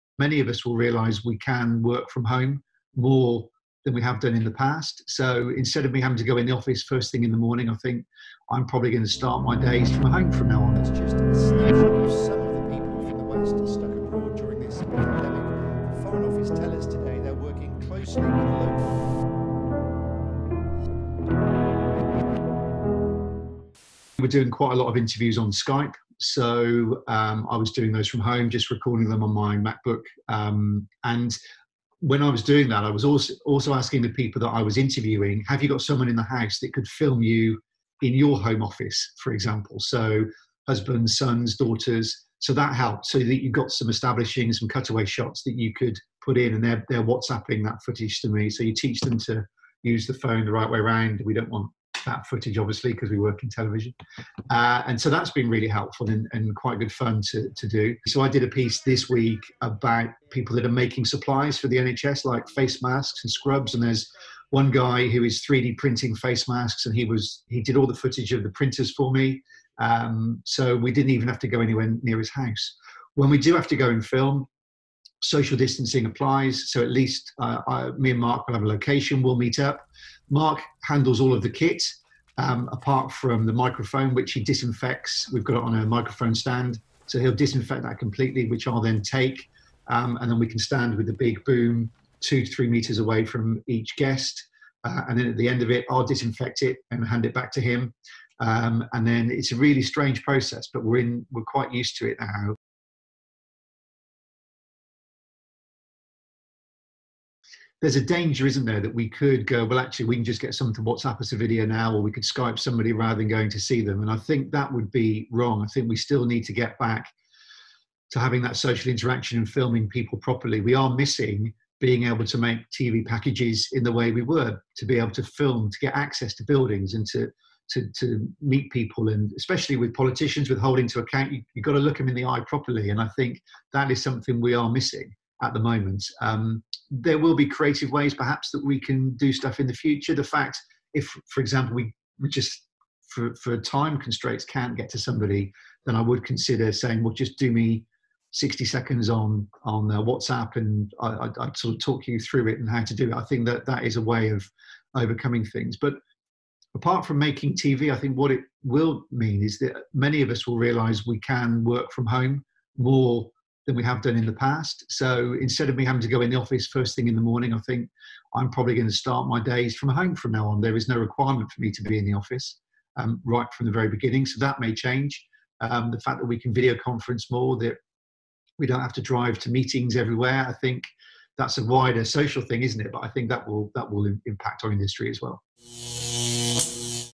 Being a TV journalist during the outbreak: Steve Knibbs, BBC
Steve Knibbs, a reporter at BBC Points West, shares his workflows and insights about working from home during the COVID-19 outbreak